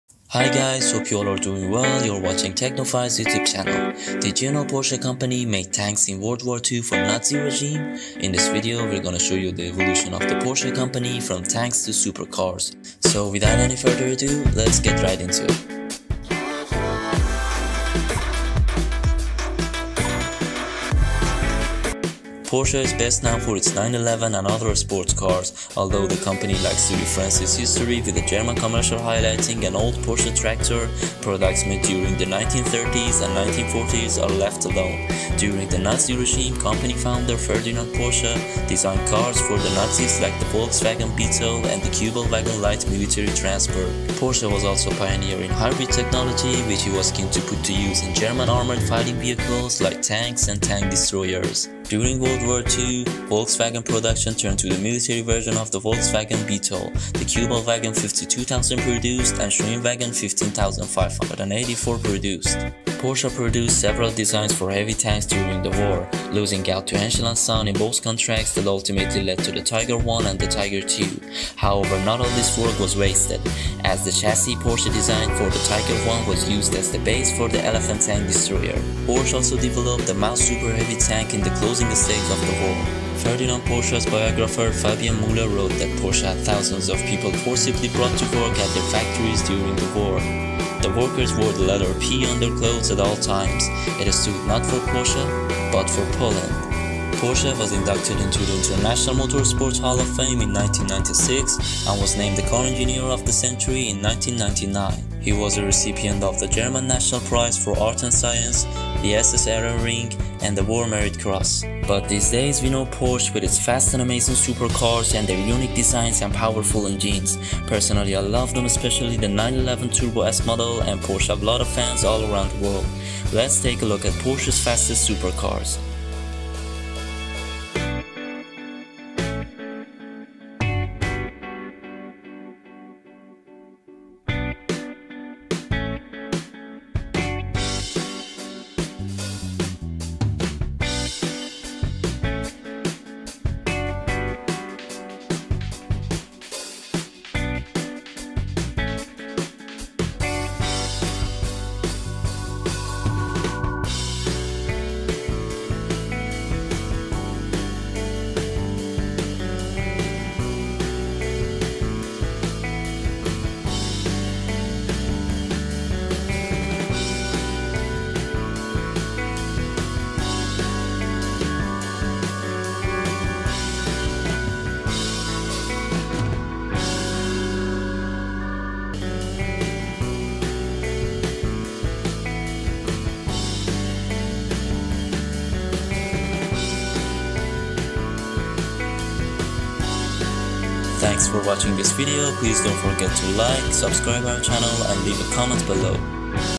PORSCHE company evolution, a history from making tanks in world war 2 to supercars /porsche
PORSCHE company evolution and history from making tanks in world war 2 to supercars
Did you know porsche company made tanks in world war II for nazi regime? In this video we're gonna show you the evolution of the porsche company from tanks to supercars.